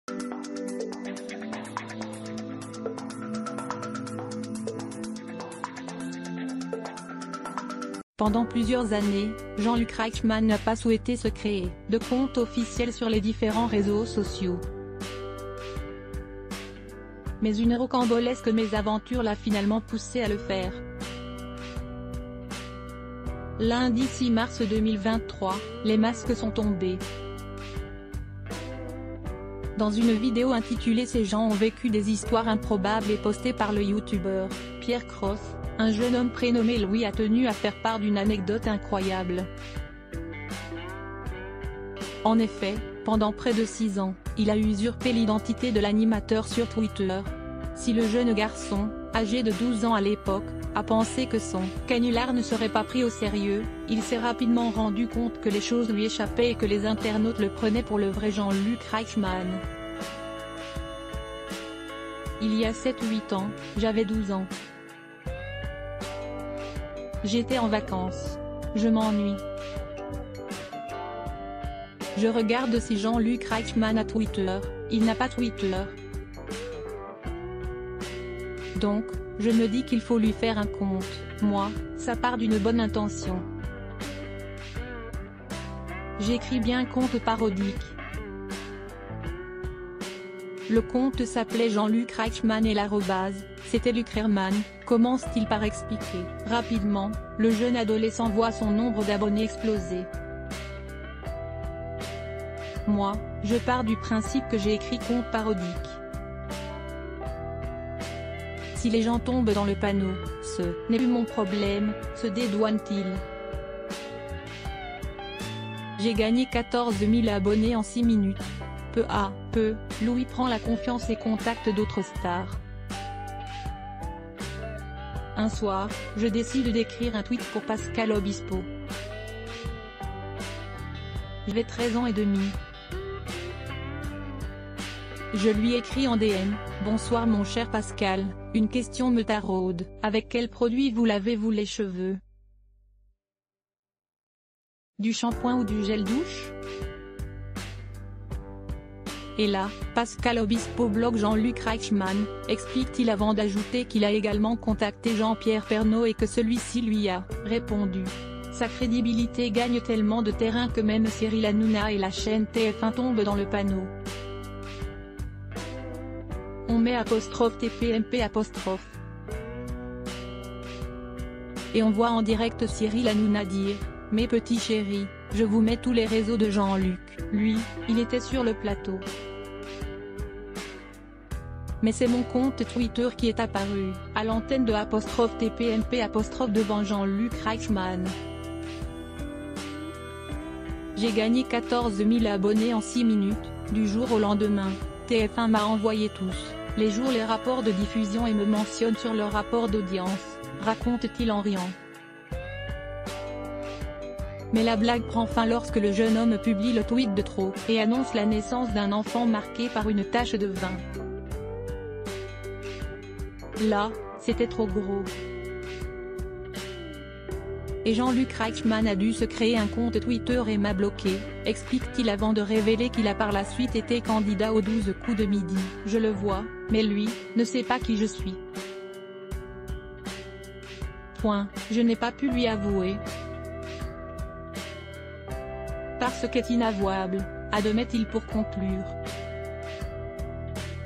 Jean-Luc Reichmann, son identité volée par un ado : des stars sont tombées dans le panneau, imp...
Jean-Luc Reichmann, son identité volée par un ado : des stars sont tombées dans le panneau, impressionnantes révélations